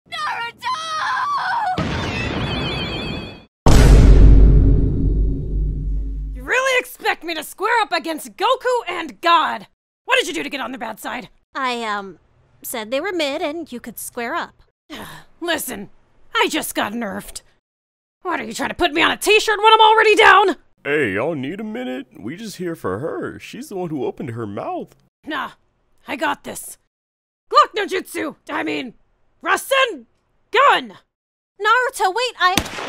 Naruto vs Goku and God
sakura tries to get naruto to go against goku and god